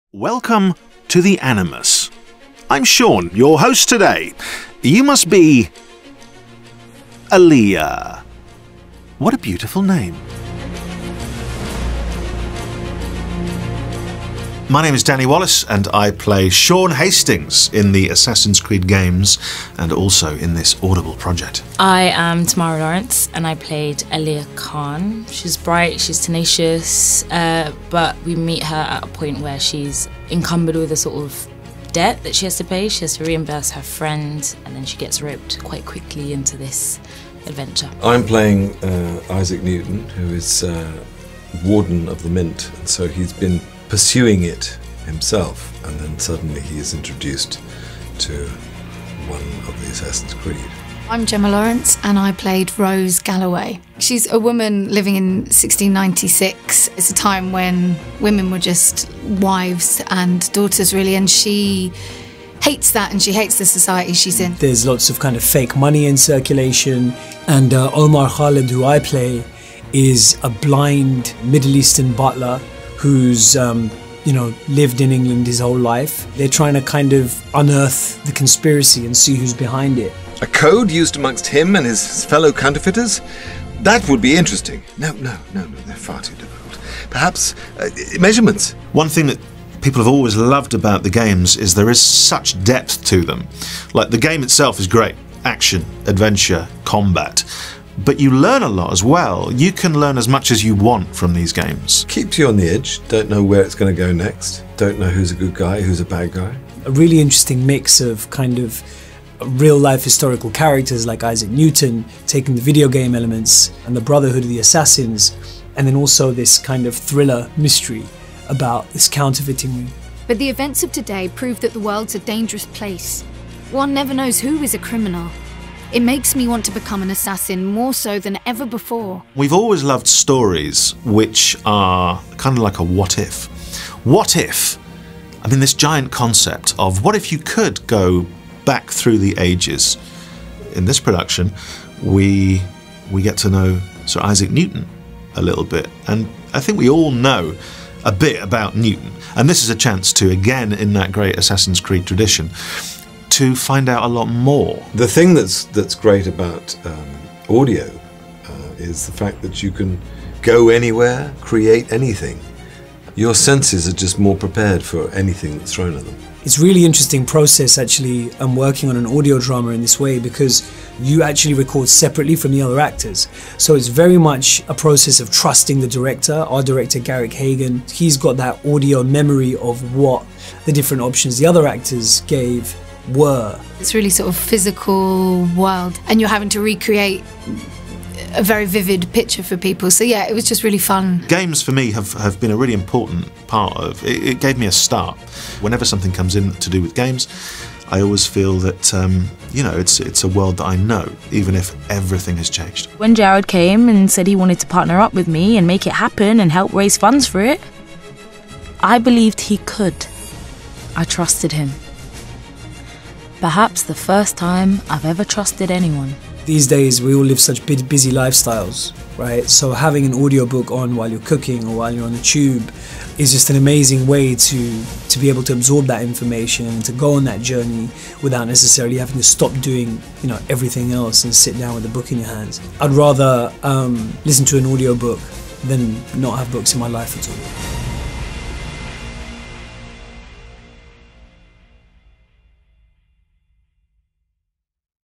Assassin's Creed: Gold - Behind the scenes with the cast | Audible UK
Embracing the best of the Assassin's Creed games’ characters, sophisticated plotting and conspiracy laden story lines Assassin’s Creed: Gold is an epic tale of imminent economic collapse, suspense and friendship set amid the eternal battle which rages between the Assassins and Templars.

This gripping audio thriller features lead performances from Riz Ahmed, Anthony Head, Danny Wallace and Tamara Lawrance alongside a full cast.